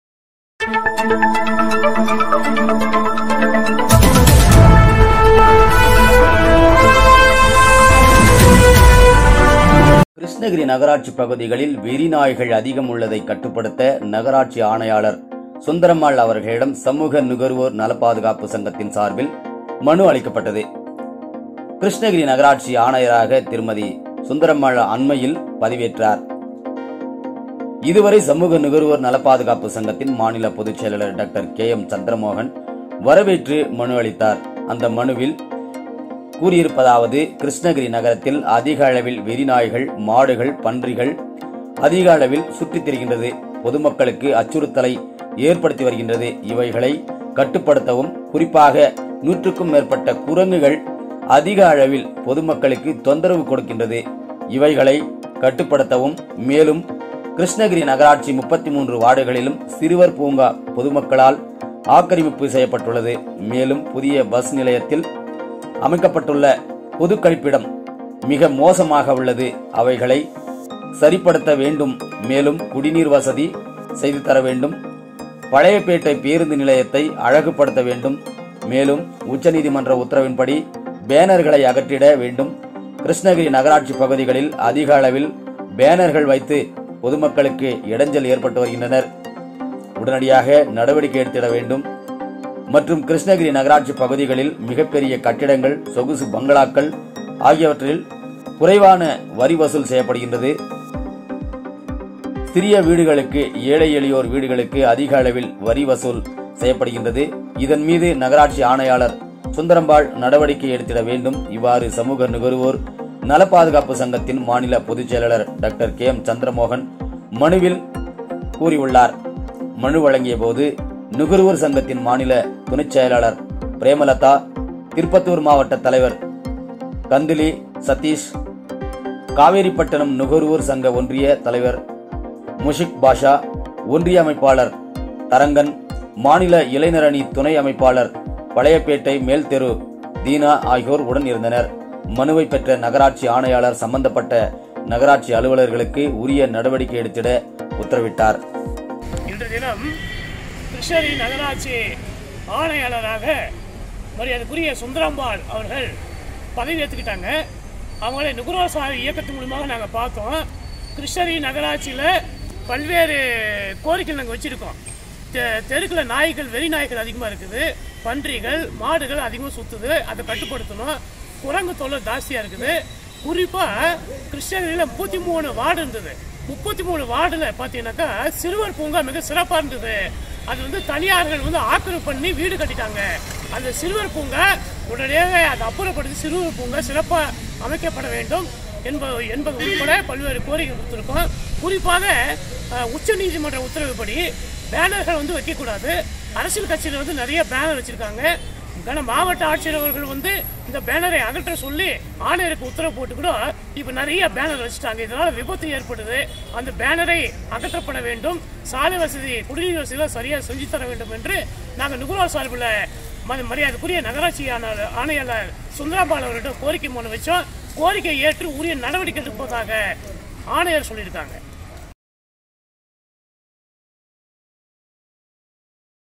கிருஷ்ணகிரி நகராட்சி பகுதிகளில் வெறி நாய்கள்அதிகம்உள்ளதை கட்டுப்படுத்த நகராட்சி ஆணையாளர் அவர்களிடம்.
02/06/2022கிருஷ்ணகிரி நகராட்சி பகுதிகளில் வெறி நாய்கள் அதிகம் உள்ளதை கட்டுப்படுத்த நகராட்சி ஆணையாளர் சுந்தராம்பாள் அவர்களிடம் சமூக நுகர்வோர் நல பாதுகாப்பு சங்கத்தின் சார்பில் மனு அளித்தனர்

 கிருஷ்ணகிரி நகராட்சி ஆணையராக திருமதி சுந்தராம்பாள் அண்மையில் பதவி ஏற்றார் இவரை சமூக நுகர்வோர் நல பாதுகாப்பு சங்கத்தின் மாநில பொதுச் செயலாளர் டாக்டர் கே எம் சந்திரமோகன் வரவேற்று மனு அளித்தார் அந்த மனுவில் கூறியிருப்பதாவது

 கிருஷ்ணகிரி நகரத்தில் அதிக அளவில் வெறி நாய்கள் மாடுகள் பன்றிகள் அதிக அளவில் சுற்றித்திரிந்து பொதுமக்களுக்கு அச்சுறுத்தலை ஏற்படுத்தி வருகின்றது இவைகளை கட்டுப்படுத்தவும் குறிப்பாக நூற்றுக்கும் மேற்பட்ட குரங்குகள் அதிக அளவில் பொதுமக்களுக்கு தொந்தரவு கொடுக்கிறது இவைகளை  கட்டுப்படுத்தவும்

மேலும் கிருஷ்ணகிரி நகராட்சி 33 வார்டுகளில் சிறுவர் பூங்கா பொதுமக்களால் ஆக்கிரமிப்பு செய்யப்பட்டுள்ளது மேலும் புதிய பஸ் நிலையத்தில் அமைக்கப்பட்ட பொதுக்கழிப்பிடம் மிக மோசமாக உள்ளது அவைகளை சரிப்படுத்த வேண்டும் மேலும் குடிநீர் வசதி செய்து தர வேண்டும் பழைய பேட்டை பேருந்து நிலையத்தை அழகு படுத்த வேண்டும் மேலும் உச்சநீதிமன்ற உத்தரவின்படி பேனர்களை அகற்றிட வேண்டும் கிருஷ்ணகிரி நகராட்சி பகுதிகளில் அதிக அளவில் பேனர்கள் வைத்து பொதுமக்களுக்கு இடைஞ்சல் ஏற்றப்படுகிறது உடனடியாக நடவடிக்கை எடுத்திட வேண்டும் மேலும் கிருஷ்ணகிரி நகராட்சி பகுதிகளில் மிகப்பெரிய கட்டிடங்கள் சொகுசு பங்களாக்கள் ஆகியவர்களுக்கு குறைவான வரி வசூல் செய்யப்படுகிறது சிறிய வீடுகளுக்கு ஏழை எளியோர் வீடுகளுக்கு அதிக அளவில் வரி வசூல் செய்யப்படுகிறது இதன்மீது நகராட்சி ஆணையாளர் சுந்தரம்பாள் நடவடிக்கை எடுத்திட வேண்டும் இவ்வாறு சமூக நுகர்வோர் நல பாதுகாப்பு சங்கத்தின் மாநில பொதுச் செயலாளர் டாக்டர் கே எம் சந்திரமோகன் மனுவில் கூறி உள்ளார்

 மனு வழங்கும்போது நுகர்வோர் சங்கத்தின் மாநில துணைத் தலைவர் பிரேமலதா திருப்பத்தூர் மாவட்ட தலைவர் கந்திலி சதீஷ் காவேரிப்பட்டினம் நுகர்வோர் சங்க ஒன்றிய தலைவர் முசிக் பாஷா  ஒன்றிய அமைப்பாளர் தாரங்கன் மாநில இளைஞர் அணி துணை அமைப்பாளர் பழைய பேட்டை மேல் தெரு தினா ஆகியோர் உடன் இருந்தனர் மனுவை பெற்ற நகராட்சி ஆணையாளர் சம்பந்தப்பட்ட நகராட்சி அலுவலர்களுக்கு உரிய நடவடிக்கை எடுத்திட உத்தரவிட்டார்...